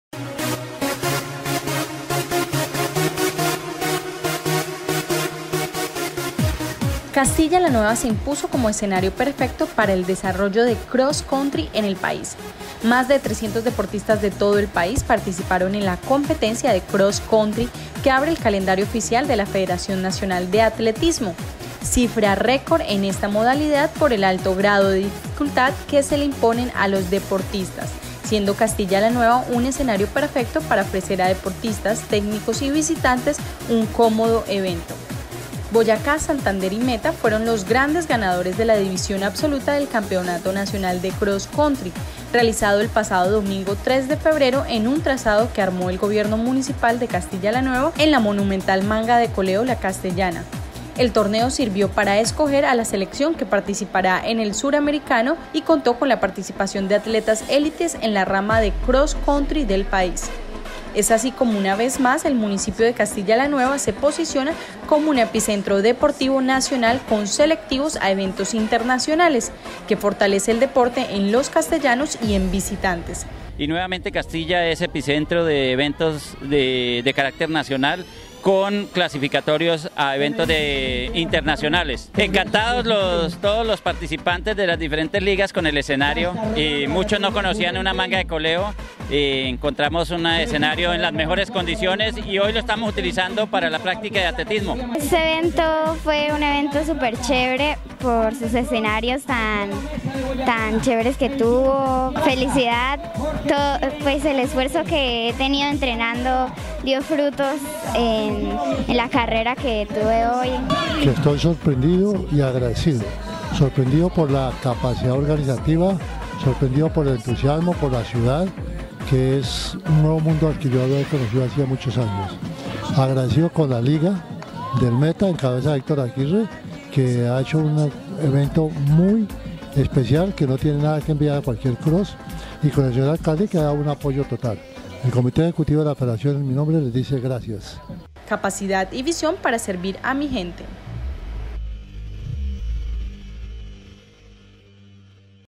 CROSS COUNTRY EN CASTILLA LA NUEVA
CrossContry | Más de 300 deportistas de todo el país, participaron en la competencia de cross country, que abre el calendario oficial de la Federación Nacional de Atletismo. Cifra record en esta modalidad por el alto grado de dificultad que se les impone a los deportistas, siendo Castilla La Nueva, un escenario perfecto para ofrecer a deportistas, técnicos y visitantes un cómodo evento.